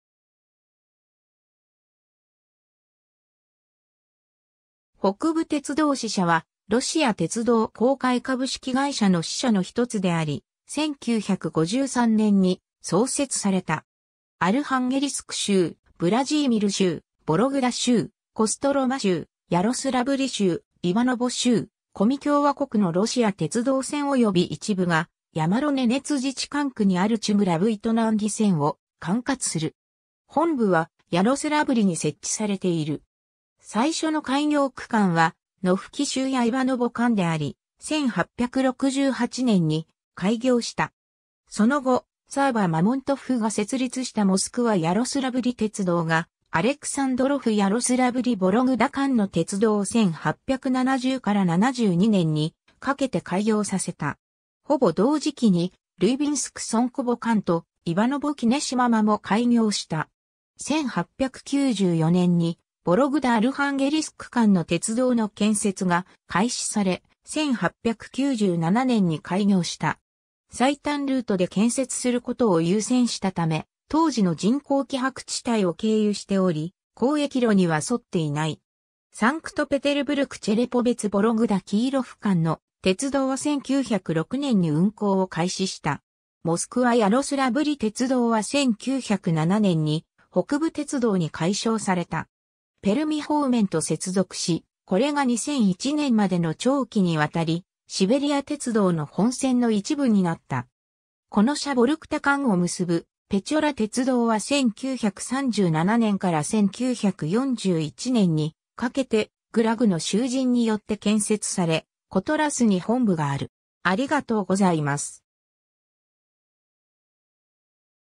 北部鉄道支社 (ロシア鉄道)
北部鉄道支社（ロシア語: Северная железная дорога）は、ロシア鉄道公開株式会社の支社（Филиал ОАО «РЖД»）の一つであり、1953年に創設された。
アルハンゲリスク州、ヴラジーミル州、ヴォログダ州、コストロマ州、ヤロスラヴリ州、イヴァノヴォ州、コミ共和国のロシア鉄道線および一部がヤマロ・ネネツ自治管区にあるチュム-ラブイトナンギ線を管轄する。
本部はヤロスラヴリに設置されている。
 最初の開業区間はノフキ - シューヤ - イヴァノヴォ間であり、1868年に開業した。
その後、サーヴァ・マモントフが設立したモスクワ・ヤロスラブリ鉄道がアレクサンドロフ - ヤロスラブリ - ヴォログダ間の鉄道を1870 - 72年にかけて開業させた。
ほぼ同時期にルイビンスク - ソンコヴォ間とイヴァノヴォ - キネシマ間も開業した。
1894年にヴォログダ - アルハンゲリスク間の鉄道の建設が開始され、1897年に開業した。
最短ルートで建設することを優先したため、当時の人口希薄地帯を経由しており、交易路には沿っていない。
 サンクトペテルブルク - チェレポヴェツ - ヴォログダ - キーロフ間の鉄道は1906年に運行を開始した。
モスクワ・ヤロスラブリ鉄道は1907年に北部鉄道に改称された。
ペルミ方面と接続し、これが2001年までの長期にわたりシベリア鉄道の本線の一部になった。
コノシャ - ヴォルクタ間を結ぶペチョラ鉄道は1937年から1941年にかけてグラグの囚人によって建設され、コトラスに本部がある。